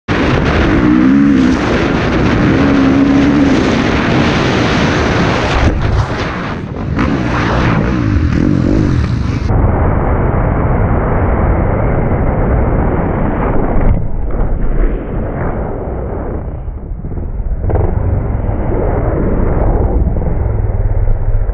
HUGE MX Crash over the bars POV!
Following a guy round mill lane mx track last year in the UK when he got ejected over the bars!